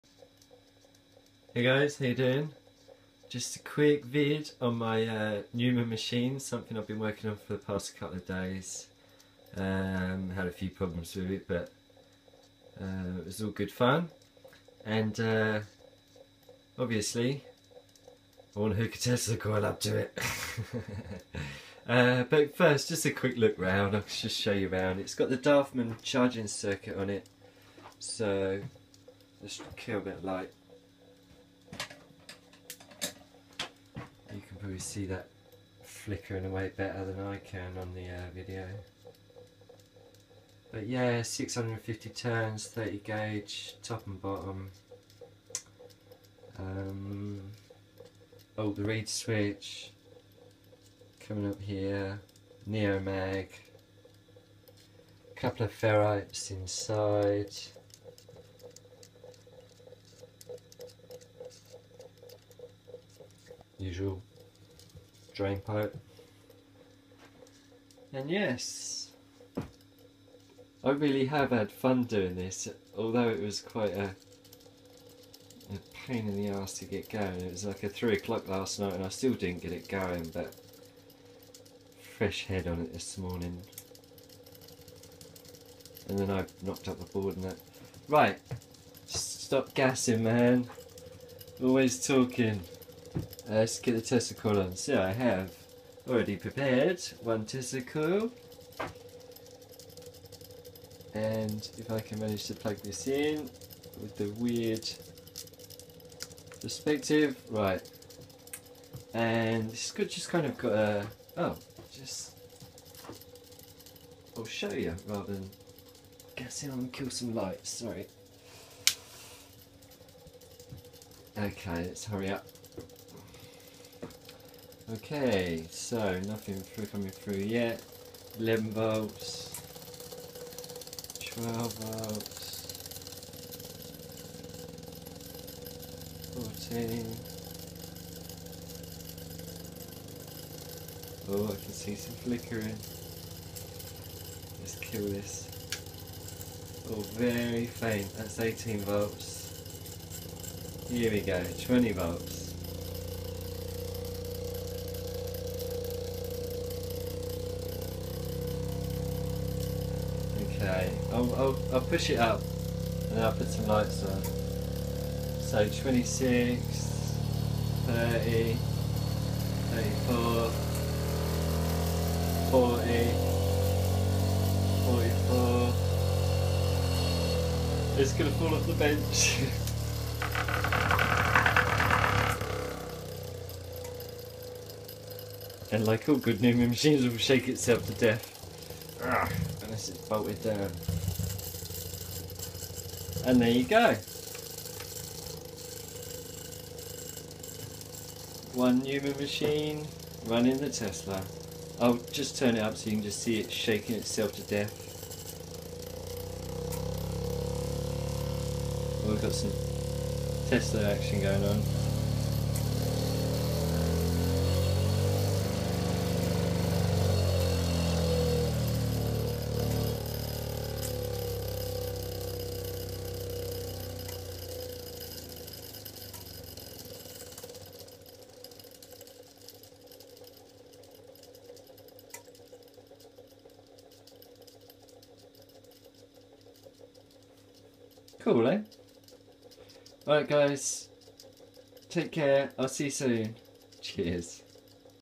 Newman Motor testing
Newman motor with the daftman switch circuit.

And I couldn't resist trying the slayer on the back end of it.